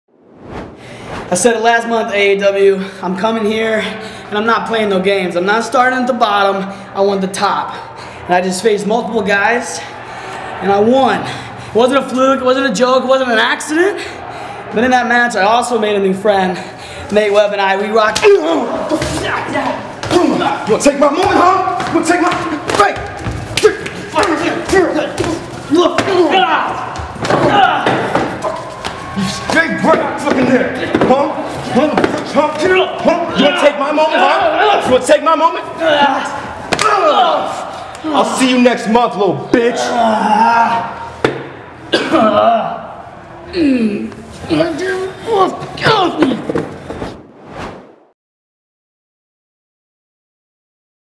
SHOCKING VIDEO - Stephen Wolf Jumps Jake Lander After The Art Of War | AAW Pro
SHOCKING VIDEO
Stephen Wolf jumps Jake Lander during his post match interview after The Art of War.